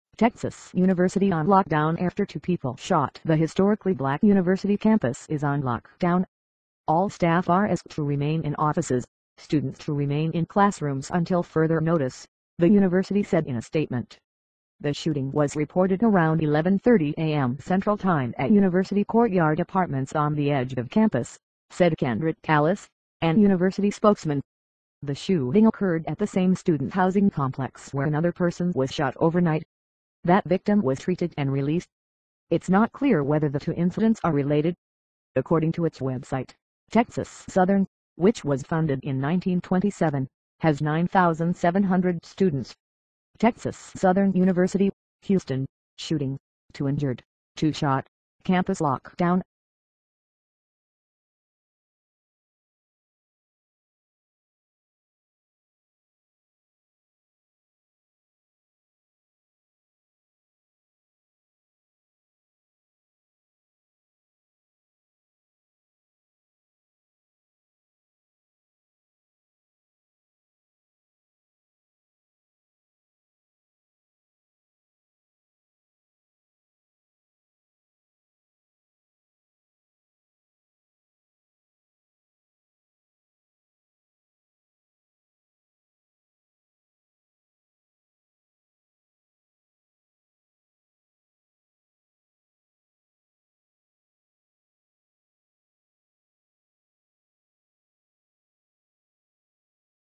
Texas university on lockdown after 2 people shot
The historically black university campus is on lockdown.

"All staff are asked to remain in offices — students to remain in classrooms until further notice," the university said in a statement.

The shooting was reported around 11:30 a.m. Central time at University Courtyard Apartments on the edge of campus, said Kendrick Callis, an university spokesman. The shooting occurred at the same student housing complex where another person was shot overnight. That victim was treated and released.

It's not clear whether the two incidents are related.

According to its website, Texas Southern, which was founded in 1927, has 9,700 students.